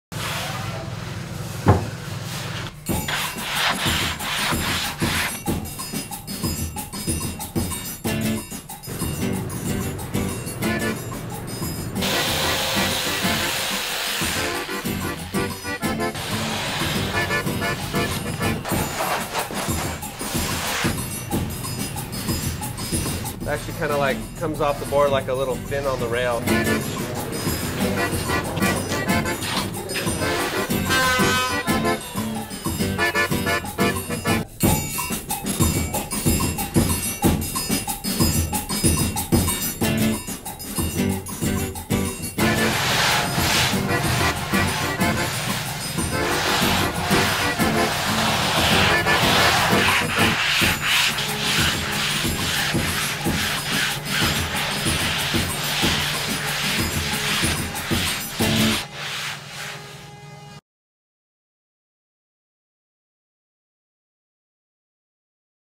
Moss Research Sustainable Surfboards
Moss Research makes high performance surfboards utilizing pine sap resin and post consumer packaging waste, removing toxins from the environment, and plastic from the landfill or ocean, before they can get there.

One minute spot directed, shot, and edited by Robert Lundahl.